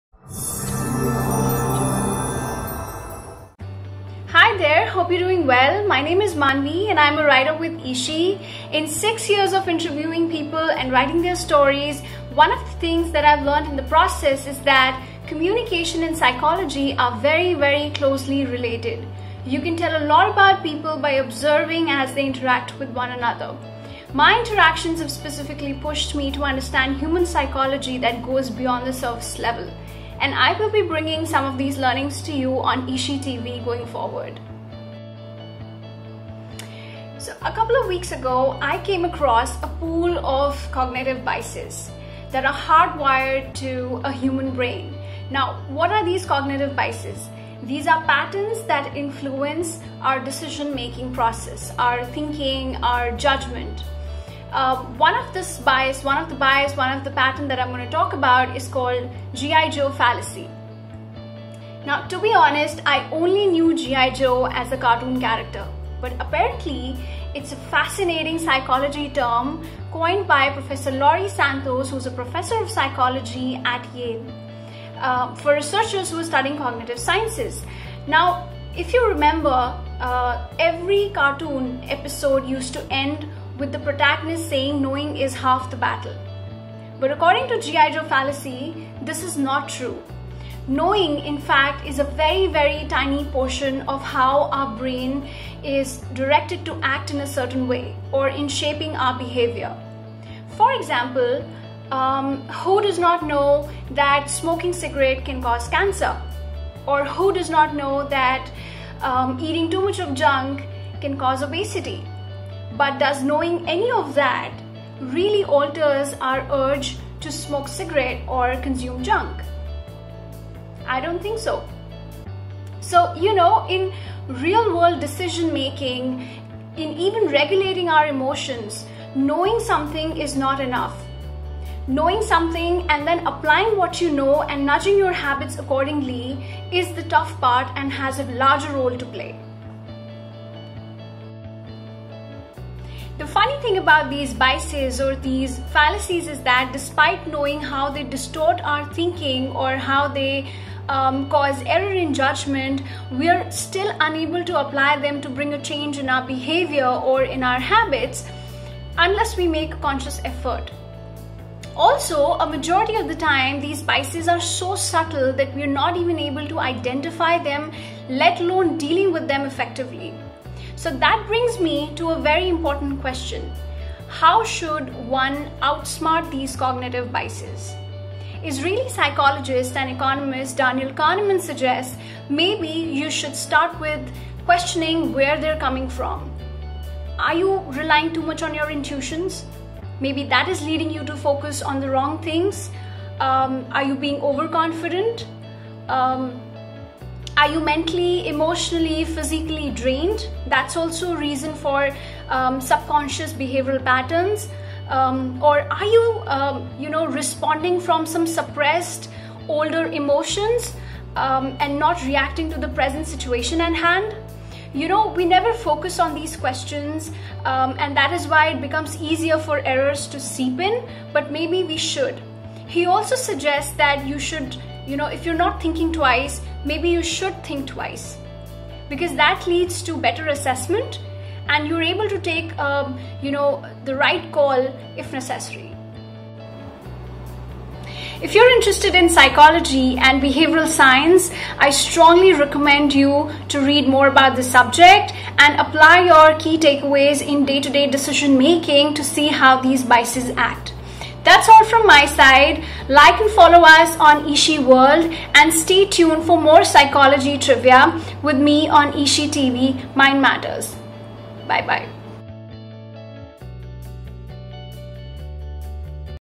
The GI Joe Fallacy and why knowing is not enough: Mind Matters with Manvi Pant on eShe TV
In eShe TV Mind Matters episode 1, Manvi Pant talks about the GI Joe Fallacy, which says that knowing something is not enough to change behaviours, and we need to take conscious action to see positive change and results.

Visit eshe.in for stories of inspirational women from around the world.